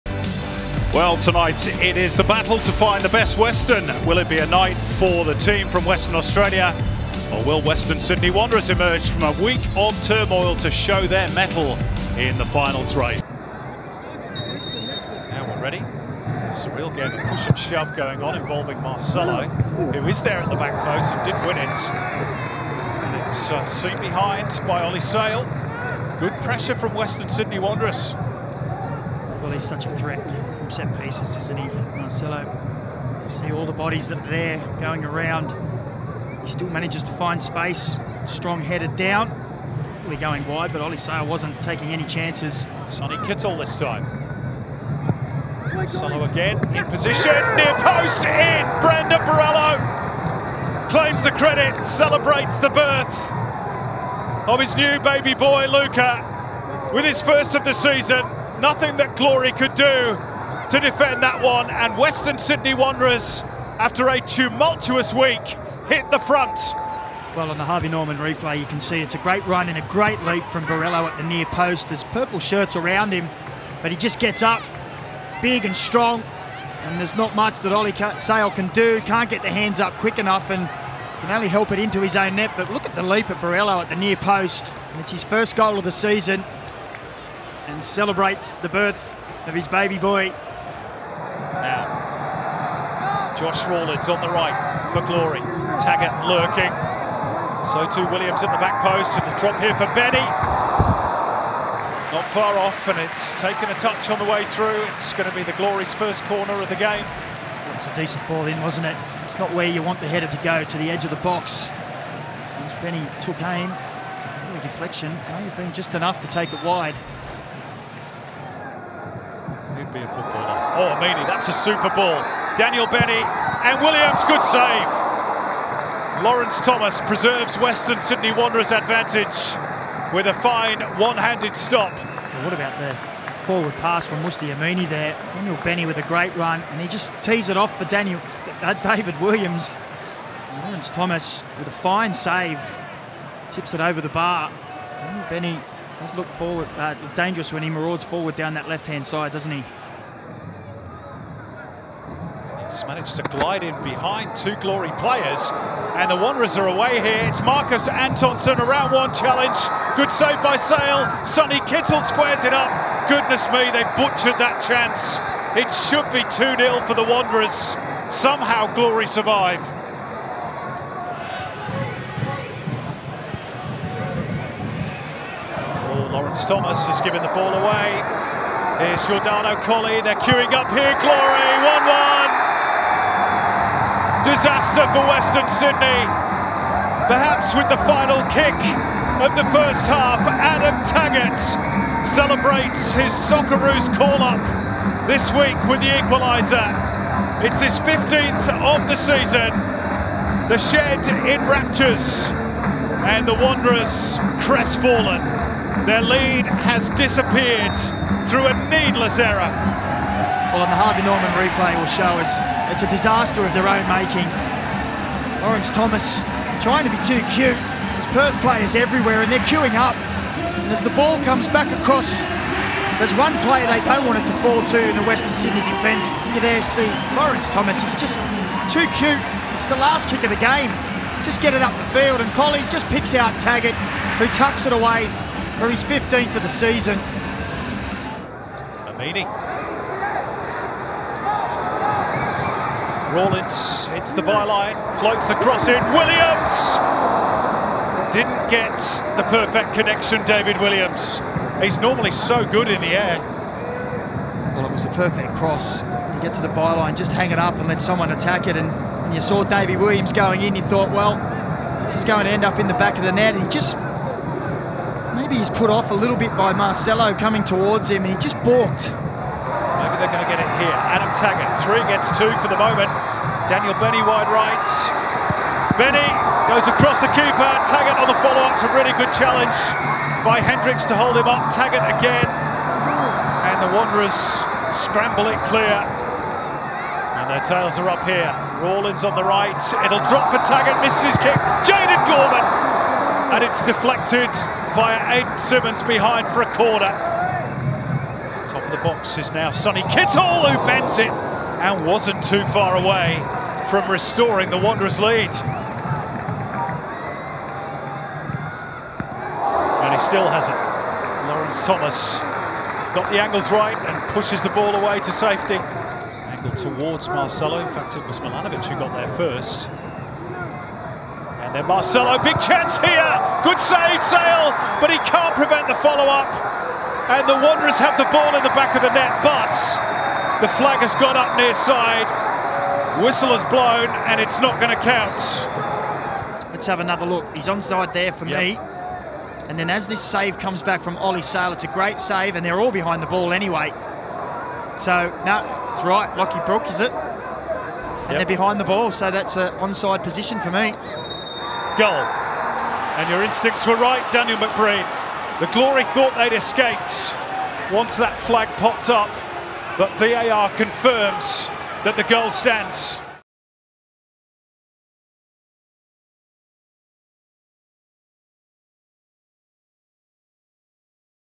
Highlights: Glory 1-2 Wanderers | Glory suffer late pain at HBF Park.
Extended highlights from match day 21 between Perth Glory and Western Sydney Wanderers.